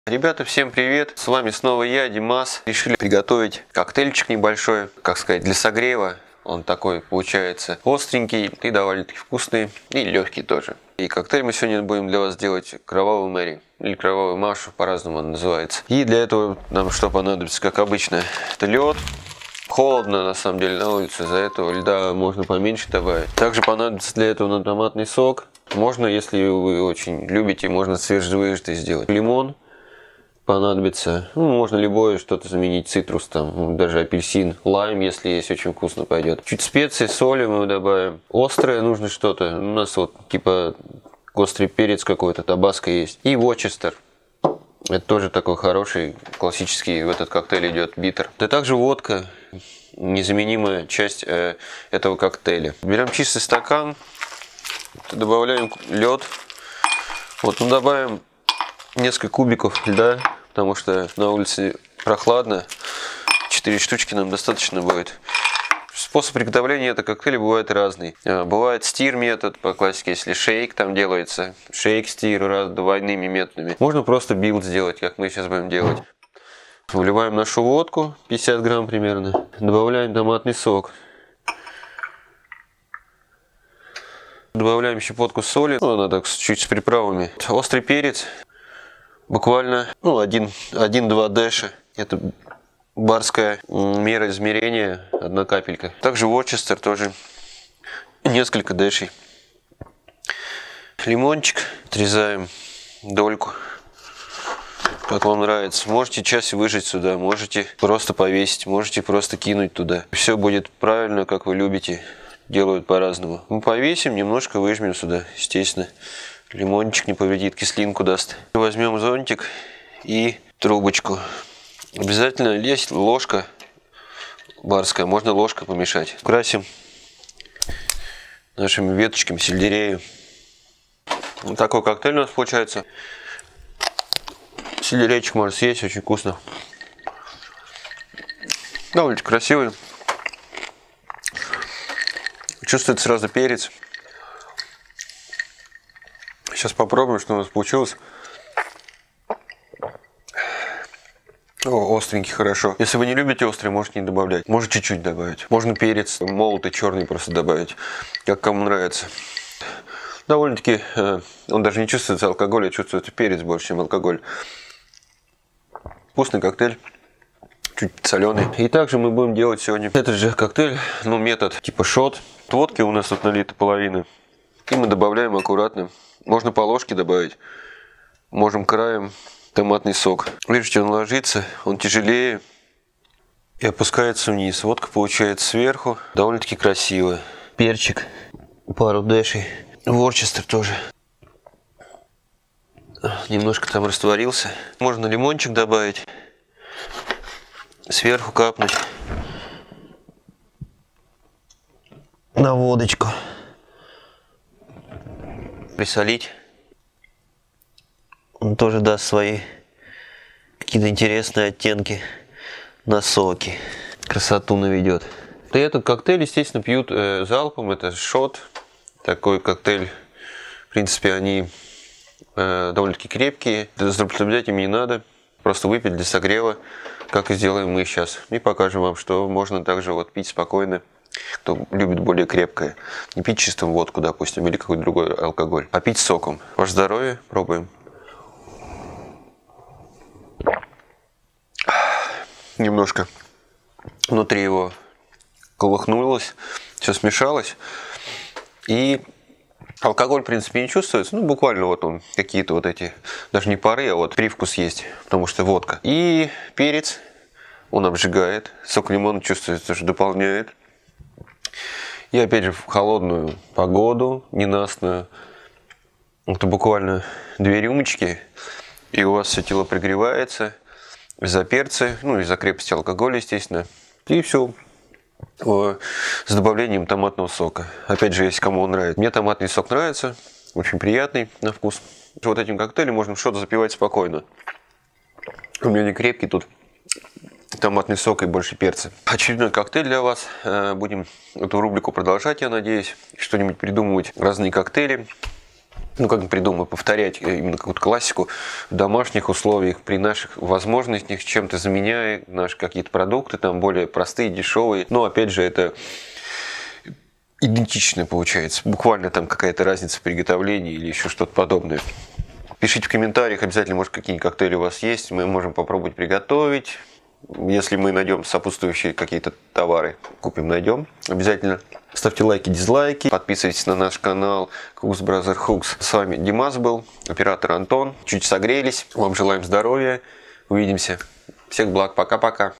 Коктейль "Кровавая Мэри " или по-русски "Кровавая Маша" Рекомендуем для согрева!
Всем привет! Сегодня мешаем классический коктейль «Кровавая Мэри» в домашних условиях.
В рецепте коктейля «Кровавой Мэри» алкогольной основой выступает водка, а не алкогольной - томатный сок . Чтобы добавить пикантности, понадобятся специи по 2-3 капли соуса Табаско и Вустерского соуса , смесь соли c приправами и не забудьте добавить сок лимона, он придаст коктейлю дополнительную яркость и раскроет вкус томатного сока. Смотрите, пробуйте, наслаждайтесь, но не злоупотребляйте алкоголем!
Для приготовления нам потребуется:
- водка;
- томатный сок;
- соус Табаско;
- соус Вустерский;
- сок лимона;
- соль;
- лёд;
- сельдерей для украшения.

И еще много интересных видеорецептов на канале!